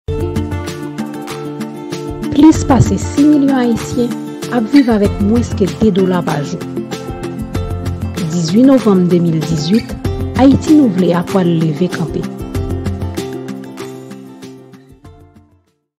Ayiti Nou Vle A - 18 novanm 2018 - Teaser 2
Dapre IHSI, PNUD, Bank Mondyal, elt, yon Ayisyen sou de ap viv ak mwens pase 2 dola pa jou

Nan AyitiNouVleA, fòk chak grenn Ayisyen.èn gen dekwa pou yo viv ak diyite.